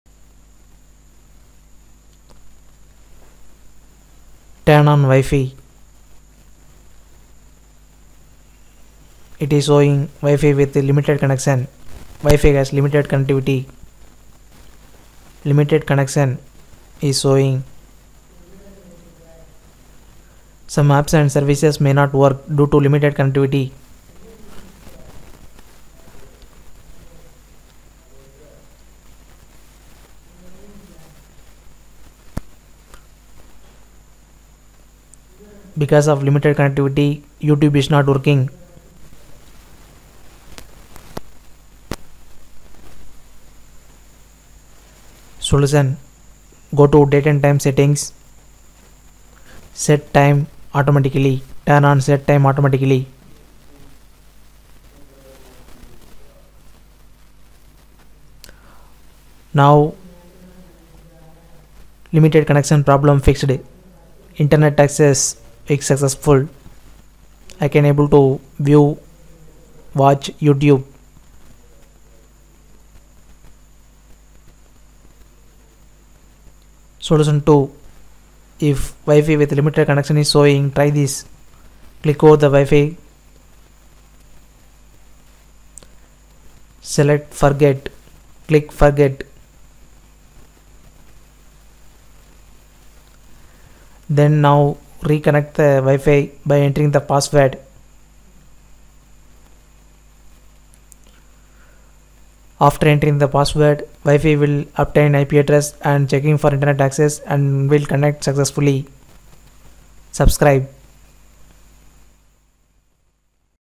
Wifi Limited Connection android fix - Wi-Fi has limited connectivity solution
When I turn on wifi it is showing limited connection. Wifi has limited connectivity. Some apps and services may not work due to limited connectivity.
wifi connected but no internet access
wifi connected limited connection